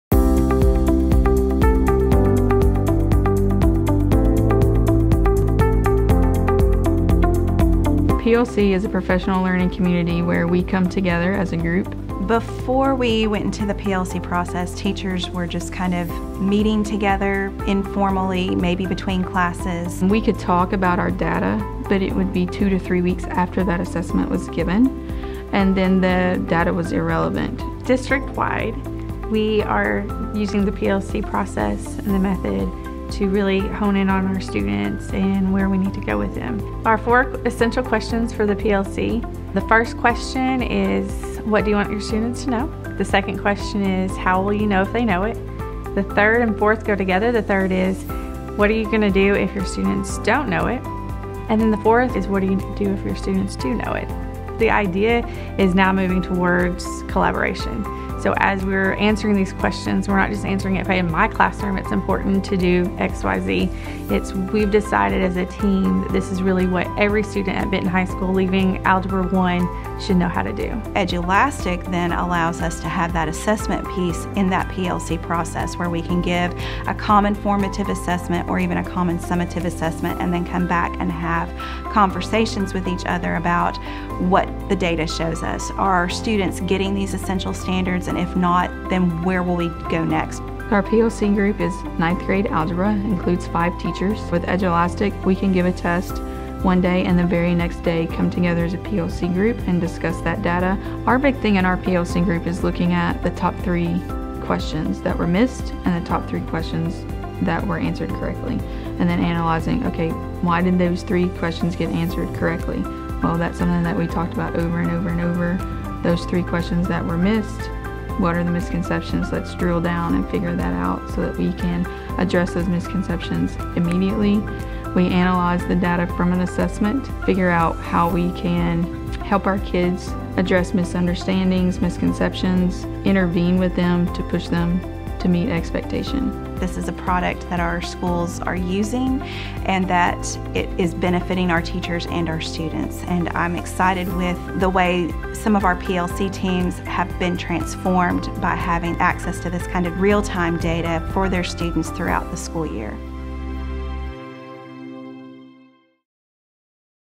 Data Fuels PLC Collaboration at Benton School District
Professional learning communities(PLCs) are a popular and growing way to bring teachers together and improve student learning. Learn about Benton School District's PLC journey and the tools that are helping them along the way.